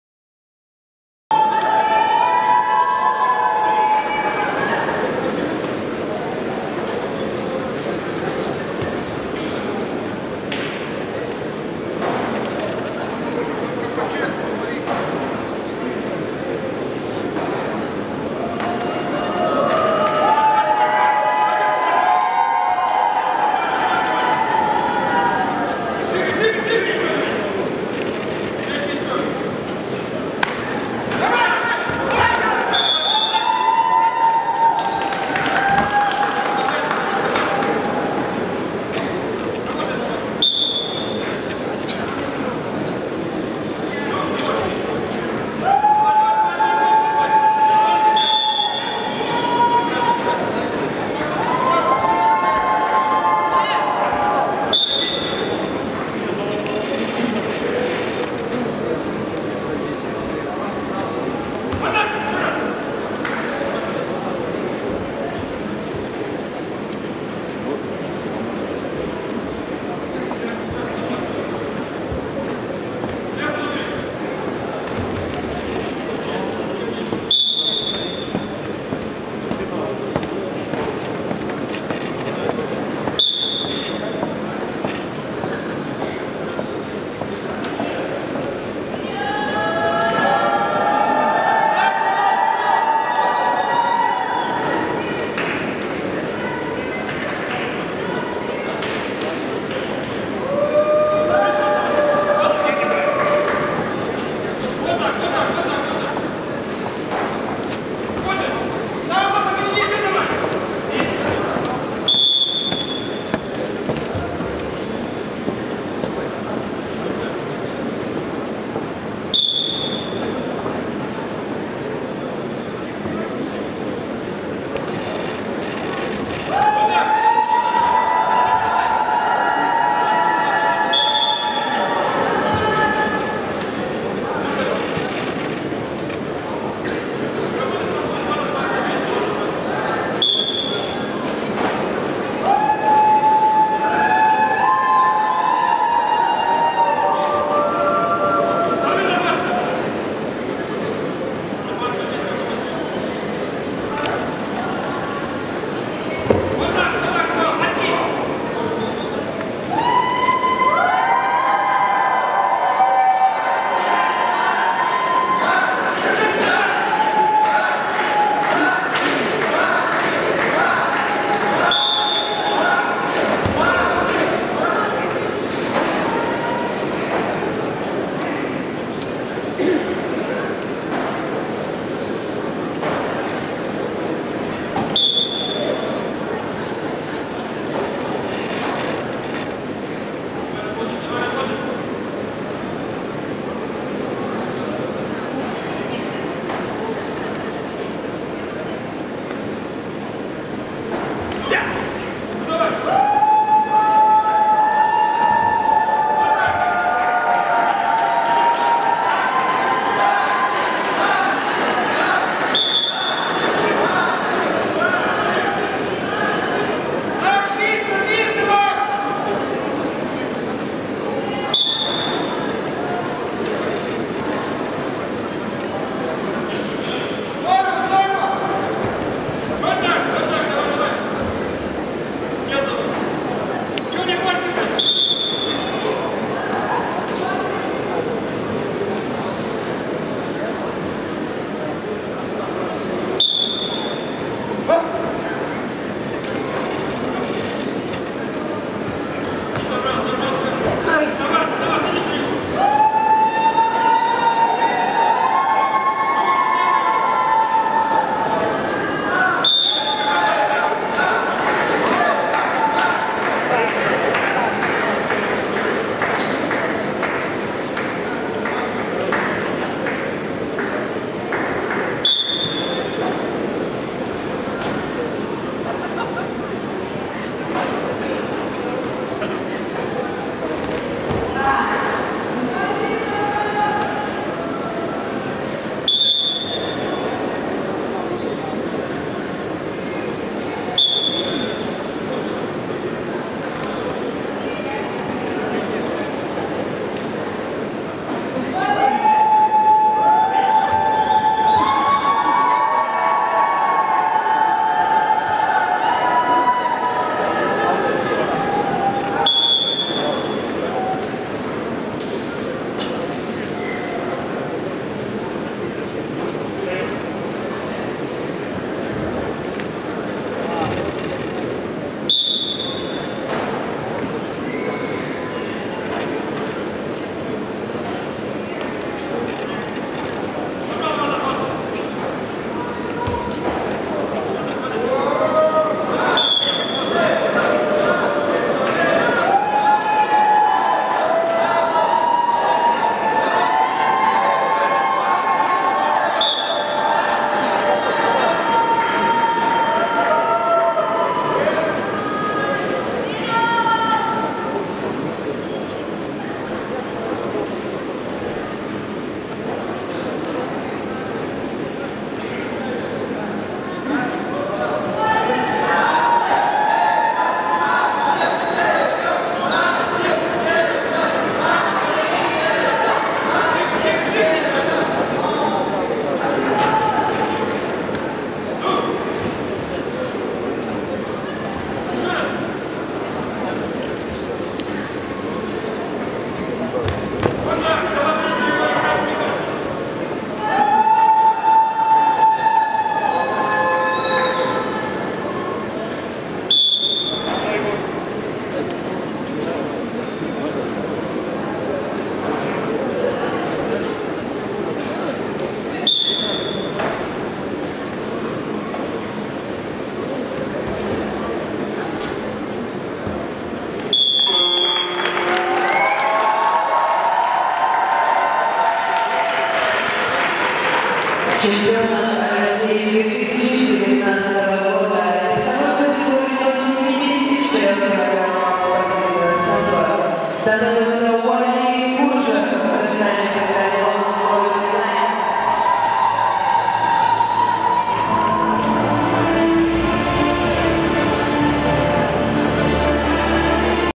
2019 САМБО финал -74 кг ЮДИН - КУРЖЕВ Чемпионат России Казань
ЮДИН Максим - КУРЖЕВ Уали
САМБО Чемпионат России 2019 Казань